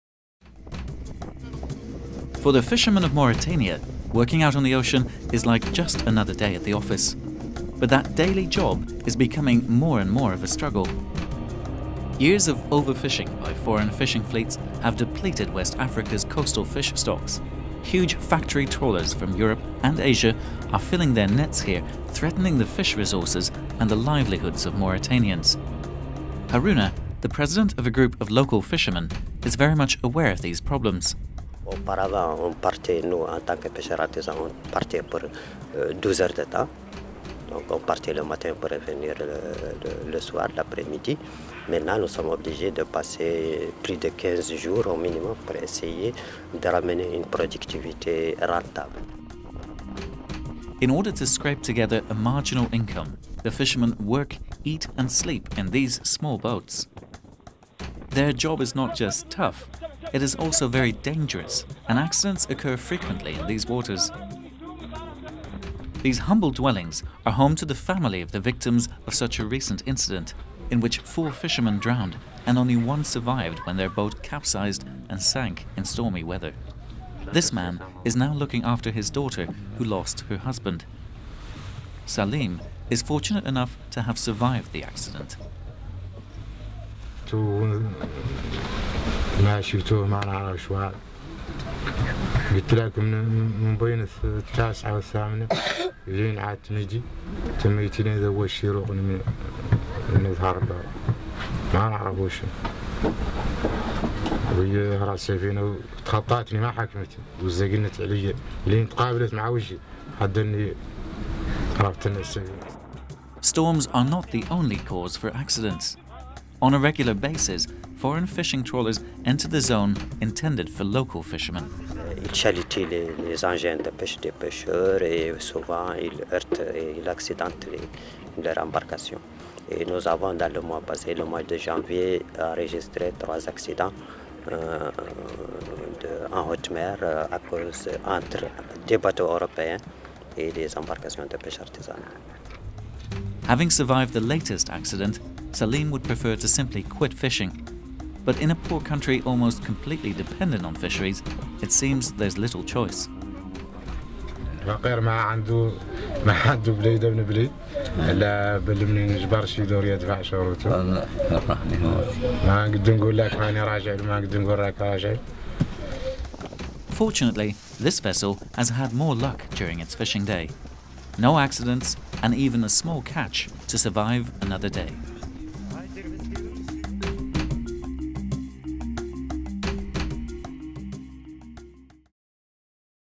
Mauritania's Risky Catch
West Africa's coastal fish stocks are being depleted by foreign fishing fleets from Europe and Asia. Overfishing is threatening the region's fish resources and the livelihoods of the people of West Africa.

For the fishermen of Mauritania, it is getting hard to make a living. Having to venture out further to sea, their small boats are at risk of capsizing in a storm or being overrun by the foreign trawlers.